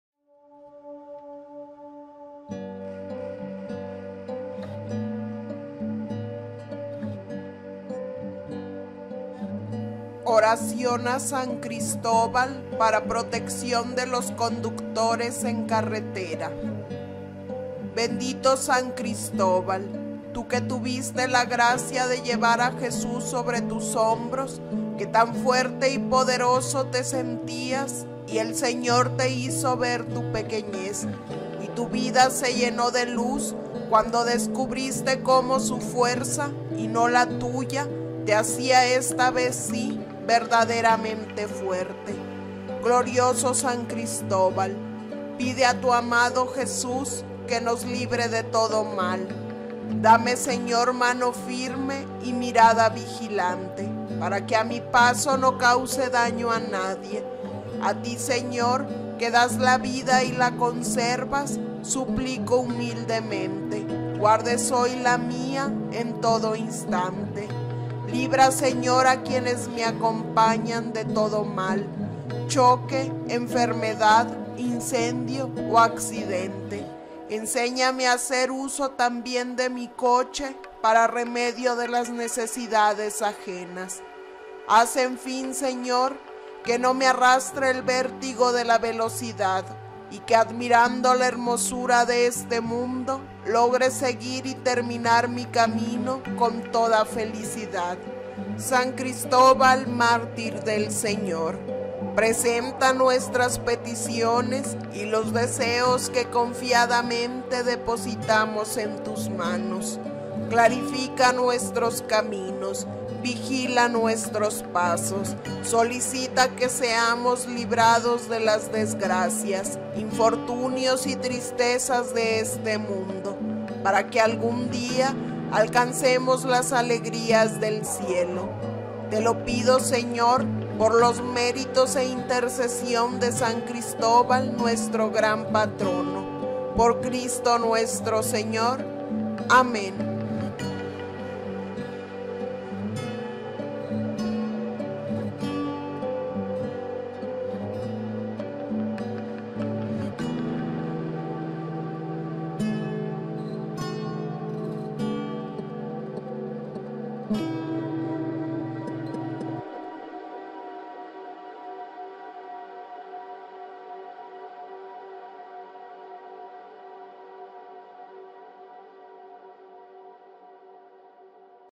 ▶ ORACIÓN A SAN CRISTÓBAL PARA PROTECCIÓN DE LOS CONDUCTORES EN CARRETERA - ORACION Y PAZ
Este 10 de julio celebramos a San Cristóbal santo patrón de los viajeros, conductores y transportistas, oremos para que proteja a todos ellos.